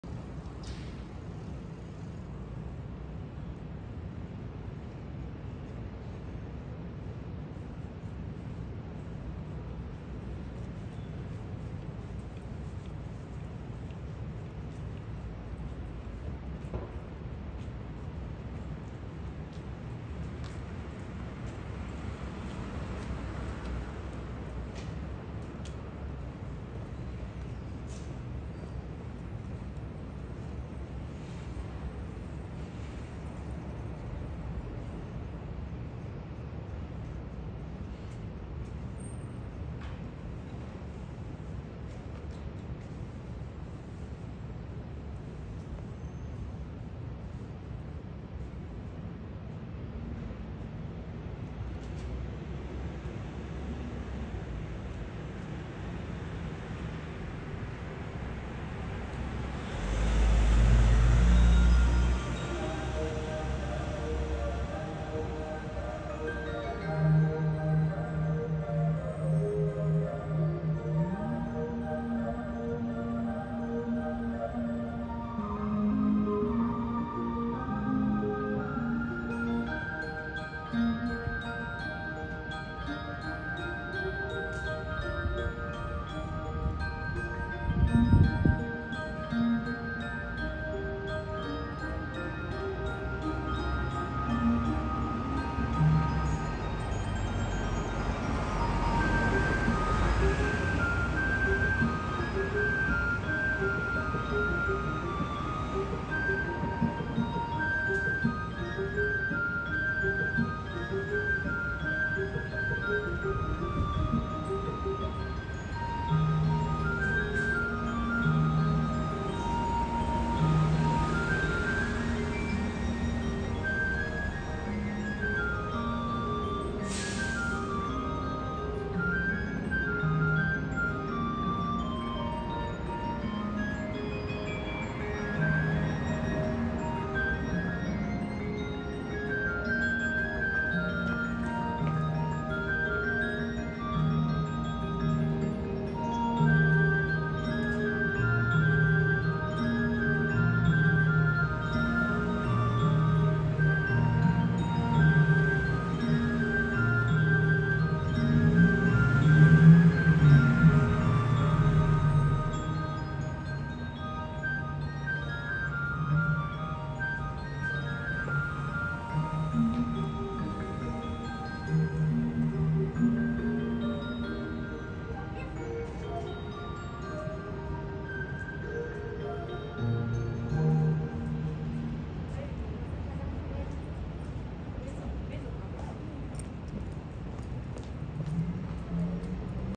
ディグ感満載！三井住友銀行相模大野支店からくり時計
2022年5月撮影。
人形など見た目のディグ感がすごいですね。
リニューアル以降しっかり状態を保っていて好感が持てます。